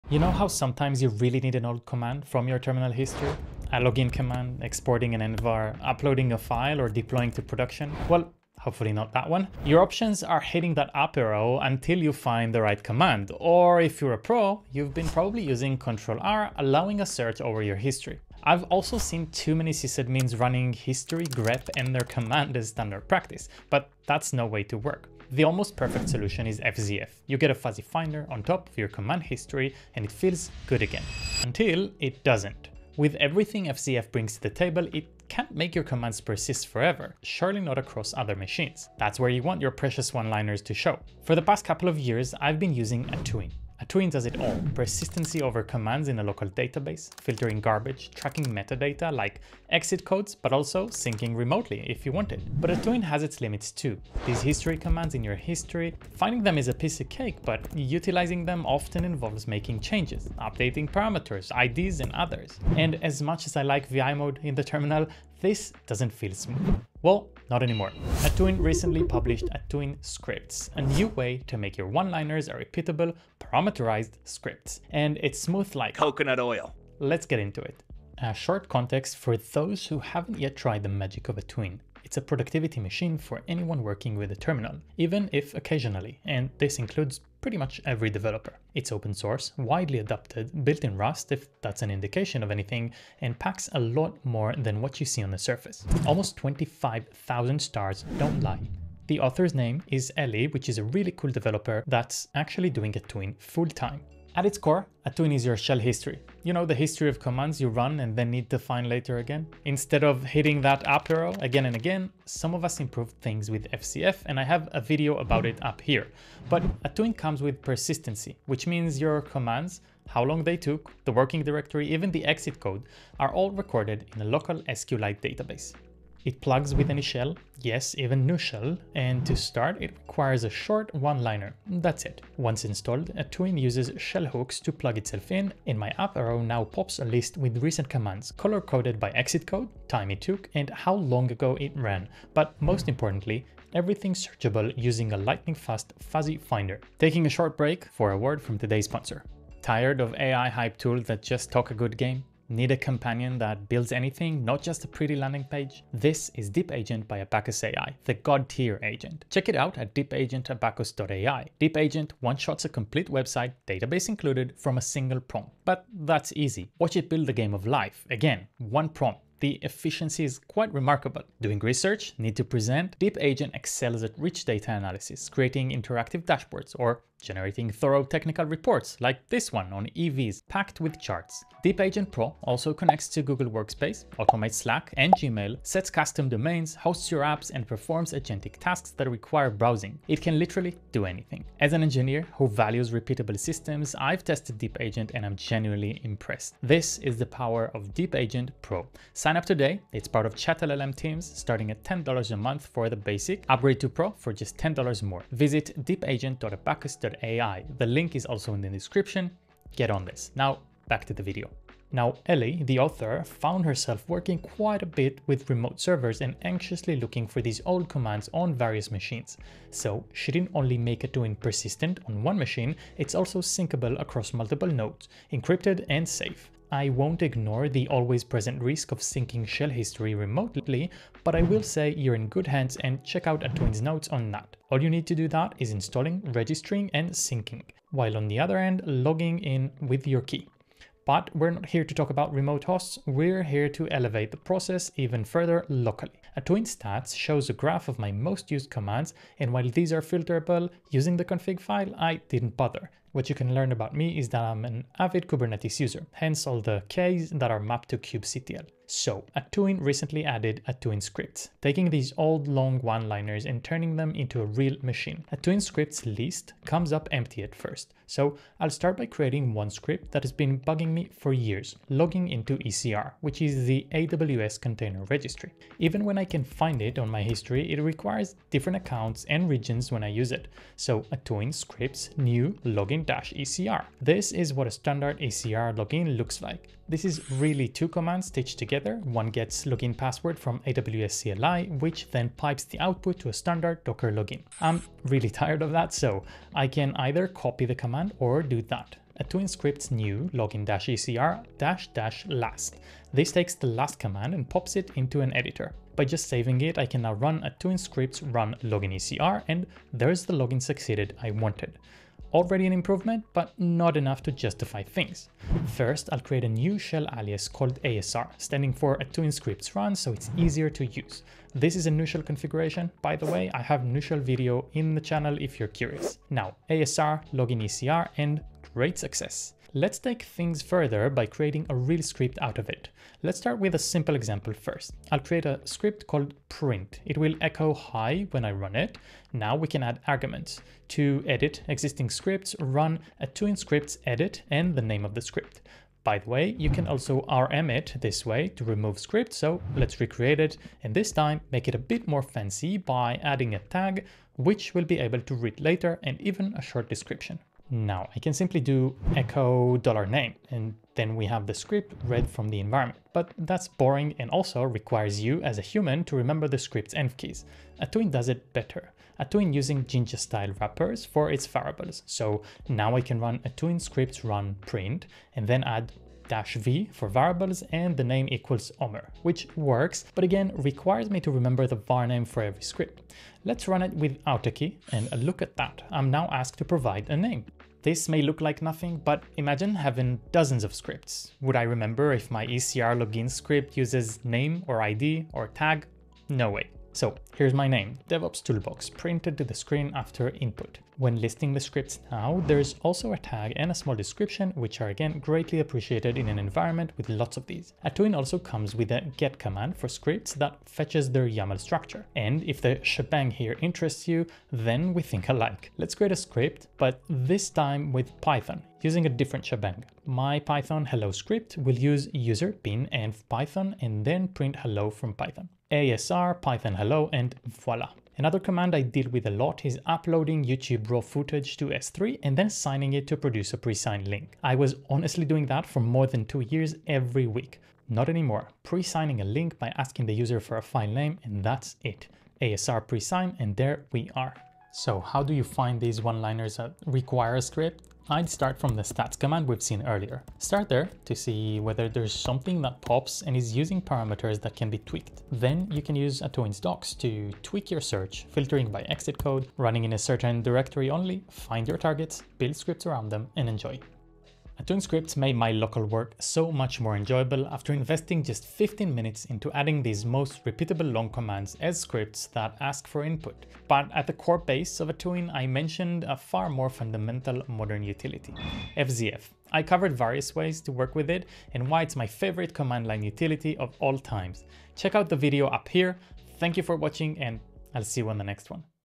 Your favourite CLI just got better.
Atuin is an amazing history fuzzy finder with persistency, and is one of my most frequently used tools. Now, it got better: Atuin scripts brings a new feature, allowing for interactive automation scripts we all have lying around, and I've been using them constantly since the release.

00:00 - Intro
02:27 - Sponsored Ad
03:27 - Setup